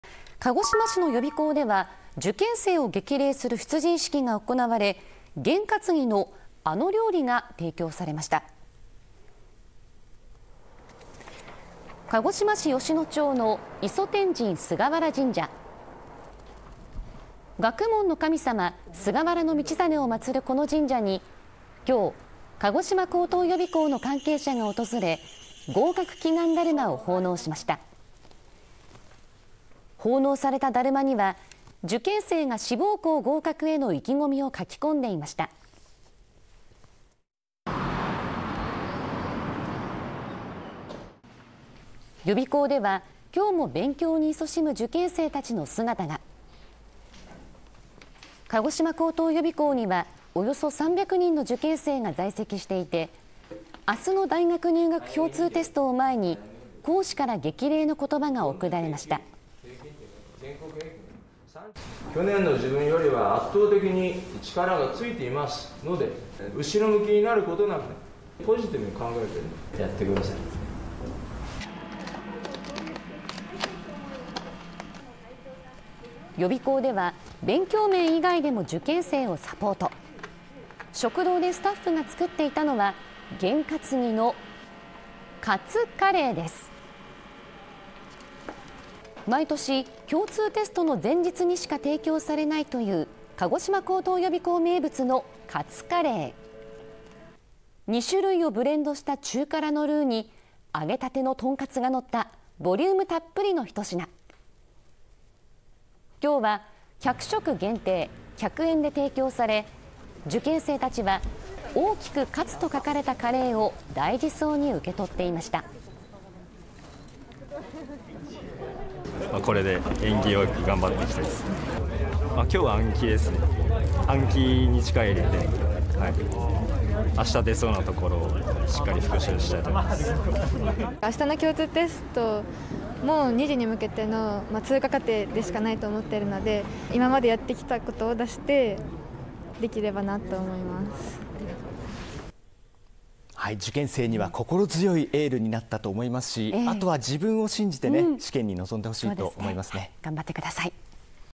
大学入試共通テスト本番目前 学問の神様に合格祈願 験担ぎの「勝カレー」で受験生応援（2023.1.13）
1月14日と翌15日は、受験生にとっては今までの勉強成果を発揮する大事な日になるわけですが、鹿児島市の予備校では受験生を激励する出陣式が行われ、験担ぎの「あの」料理が提供されました。

鹿児島市吉野町の磯天神菅原神社。

1月13日、学問の神様として知られる菅原道真公を祭るこの神社に、鹿児島高等予備校の関係者が訪れ、合格祈願だるまを奉納しました。

奉納されただるまには、受験生が志望校合格への意気込みを書き込んでいました。

予備校では、1月13日も勉強にいそしむ受験生たちの姿が。

鹿児島高等予備校には約300人の受験生が在籍していて、1月14日の大学入学共通テストを前に、講師から激励の言葉が送られました。

講師
「前年より力はついている。後ろ向きにならず、前向きに」

予備校では、勉強面以外でも受験生をサポート。

食堂でスタッフが作っていたのは、験担ぎの「勝カレー」です。

毎年、試験の前日にしか提供されないという鹿児島高等予備校名物のカツカレー。

２種類をブレンドした中辛のルーに、揚げたてのトンカツがのったボリュームたっぷりの一品。

1月12日は100食限定、100円で提供され、受験生たちは大きく「勝」と書かれたカレーを大事そうに受け取っていました。

受験生
「去年の自分よりは圧倒的に力がついているので、後ろ向きになることなくポジティブに考えてやってください」
「勝カレーで縁起良く頑張っていきたい。きょうは暗記。暗記に力を入れて、あす出そうなところをしっかり復習したい」
「あしたの共通テストは2次試験に向けての通過過程。今までやってきたことを出せれば」